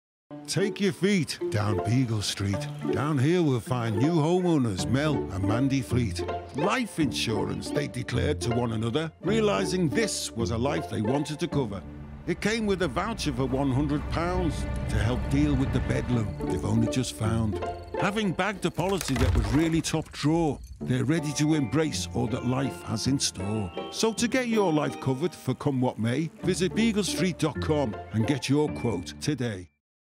Beagle Street TV Advert "First Night"- Jan 2021
Beagle Street are celebrating the launch of two new TV adverts! This ad features a young couple, Mel and Mandy who have just moved into their first home together and have an unexpected surprise waiting for them! Luckily they can rely on Beagle Street for some peace of mind.
Beagle Street, Insurance For Life.